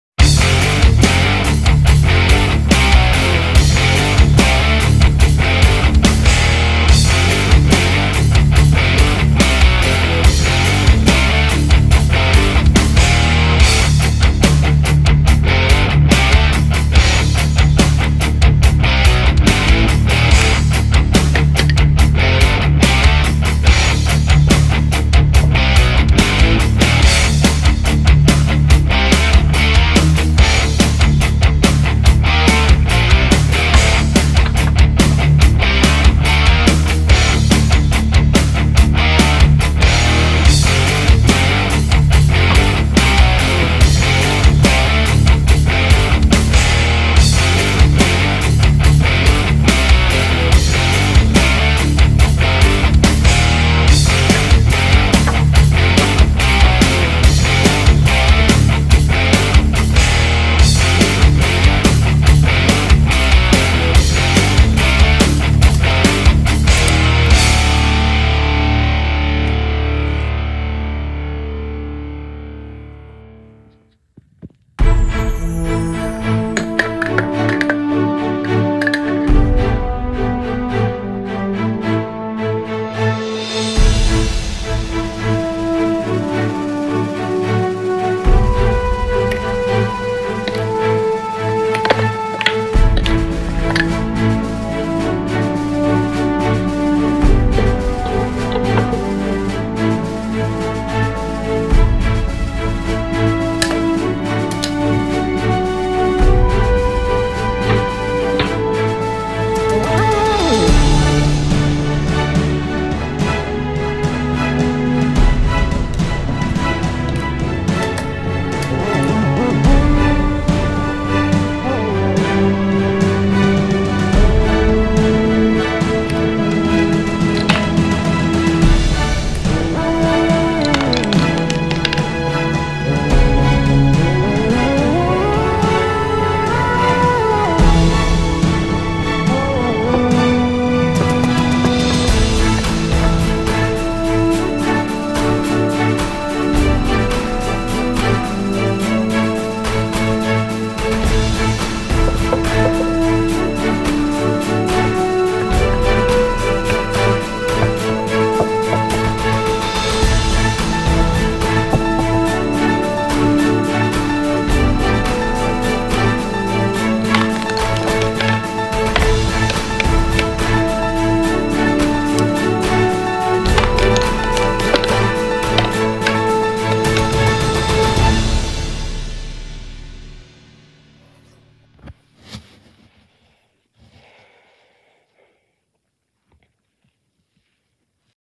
INSANE PARKOUR MARBLE RUN
This run has 3 different paths for the marbles to run on. It took about 1 hour to make, so not too long.